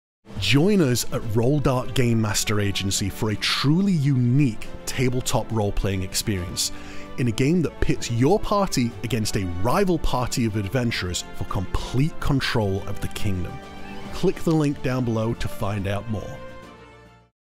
The Warring Princes - Party vs Party D&D
The Kingdom of Elsthefar stands on the verge of civil war. Last week King Alexander died leaving no clear heir to the throne, and his twin sons Alistair and Jonathan both insist that they are the rightful successor. To make matters more complicated the ancestral seal that has confirmed Royal Succession for generations has gone missing, leaving neither of the sons with any more a claim than the other to the throne. Whilst royal envoys bicker and argue in court about the rightful ruler of the kingdom, one solution has privately occurred to both princes: if one brother were to best the other in combat their lineage would be secured and they would be the new king.

As the princes prepare for war, they require allies from among their father's former subjects. Your party has seven weeks to gather as many allies as they can, to bring an army to bear for the coming storm and secure the future of the kingdom. The problem? Another band of adventurers is trying to do the exact same thing!

Join us for a truly unique adventure, as your party adventures across the country trying to win the favour of Ducal Lords, Mercantile Emperors and Powerful Mages. As you adventure, however; another party will be working against you, trying to recruit the same allies and undermining your efforts to secure victory. This game offers a truly open-world experience where you can choose your plan of action, deciding the best way to build your army. Will you travel from region to region, earning the fealty of the local rulers? Or will you try and find the Ancestral Seal in an attempt to unify the kingdom under your banner? Instead, you could spend time sabotaging the efforts of your opponents, spreading discontent in their ranks and weakening their militia, the choice is entirely up to you! The only thing that is certain is that in seven weeks time you will face off against your foes in a vicious final battle. Your party and allies on one side, another party and their allies on the other. And there can be only one victor!